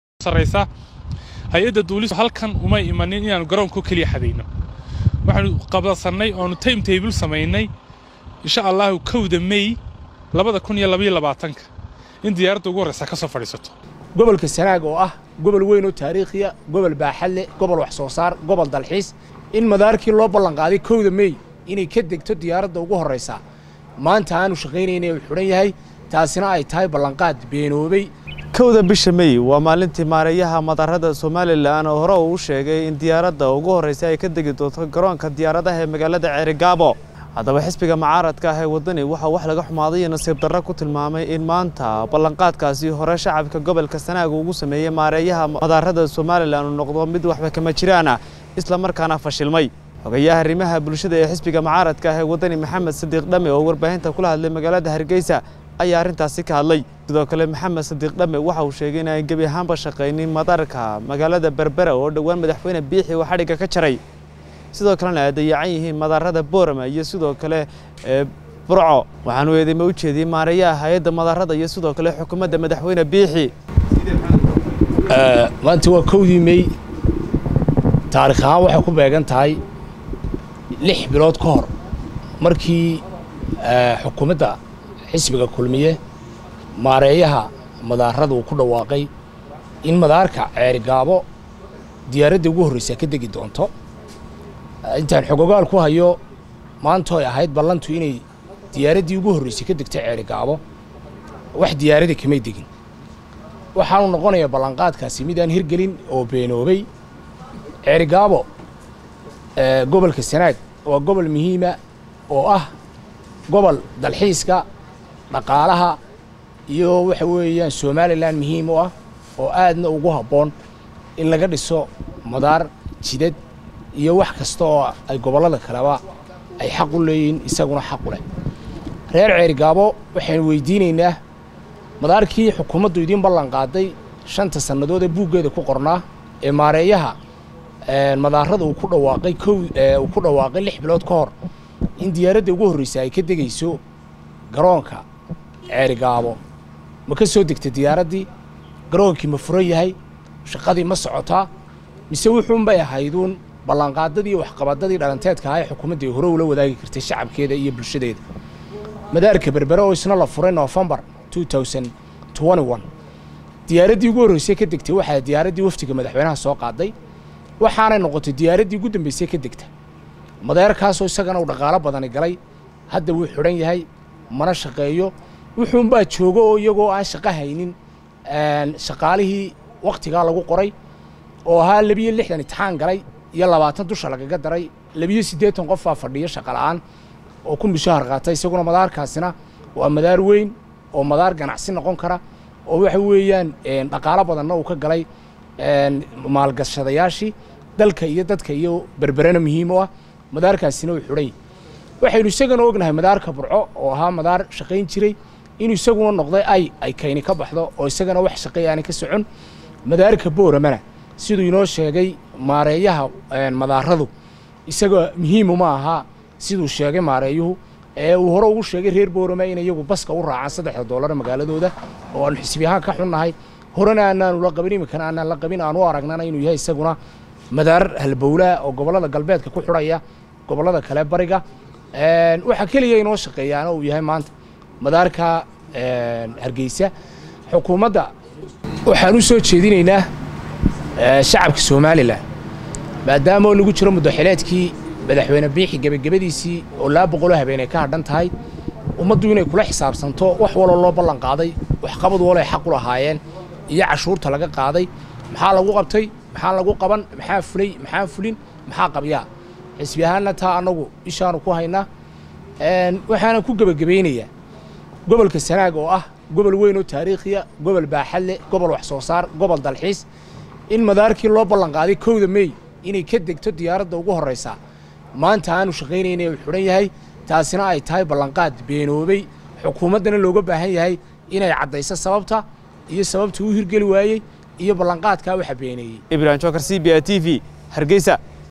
Balan qaad reer Sanaag loo qaaday oo beenoobay!
''Balanqaadkii Maareeyaha Madaaraddu u qaaday reer Sanaag in 1da May 2022 diyaaradii u horeysay ka degeyso wuu beenoobay'' Xisbiga Waddani